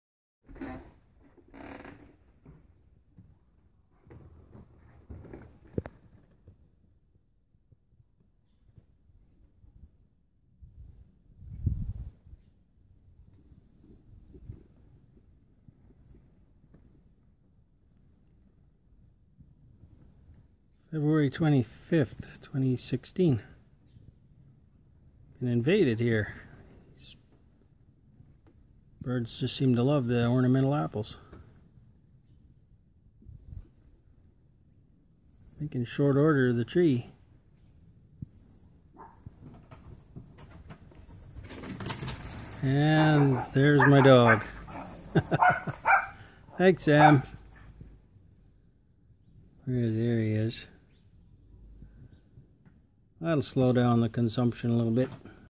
Cedar Wax wings in the ornamental apple tree.
Dozens of wax wings feast on the ornamental apple tree in our backyard in late February until our little terrier Sam decides to visit.